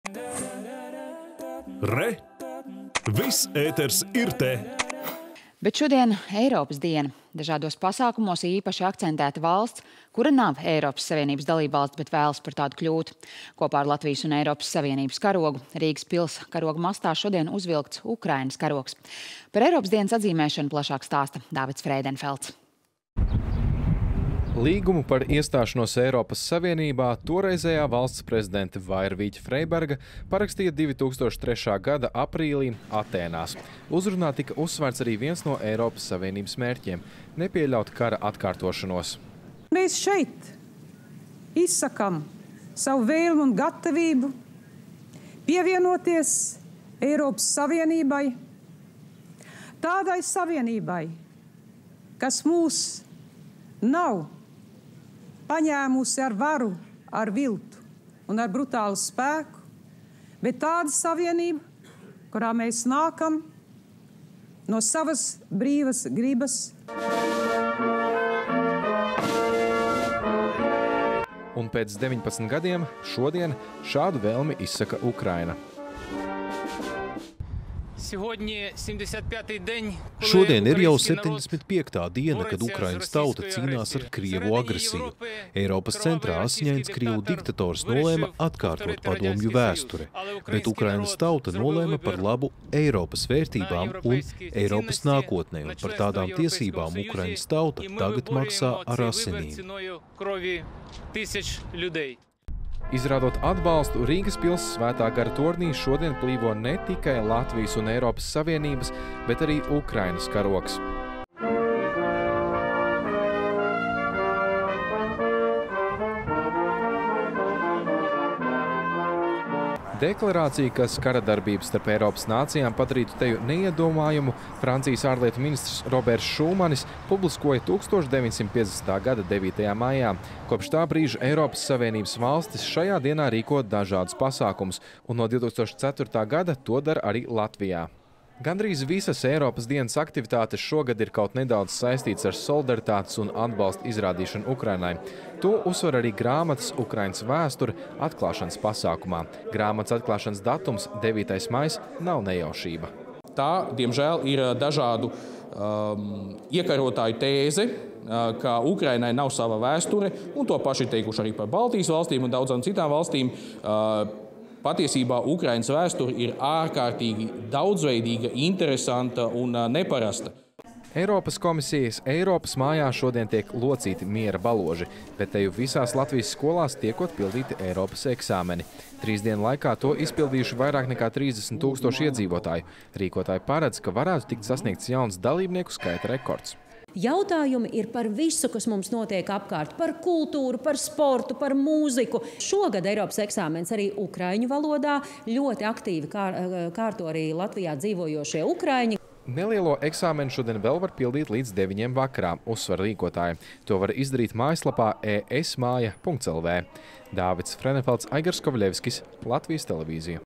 Eiropas dienā virs Rīgas plīvo arī Ukrainas karogs
Šodien – Eiropas diena. Dažādos pasākumos šoreiz īpaši akcentēta valsts, kura nav Eiropas Savienības dalībvalsts, bet vēlas par tādu kļūt. Kopā ar Latvijas un Eiropas Savienības karogu Rīgas pils karoga mastā uzvilkts arī Ukrainas karogs. Par Eiropas dienas atzīmēšanu plašāk stāsta Dāvids Freidenfelds.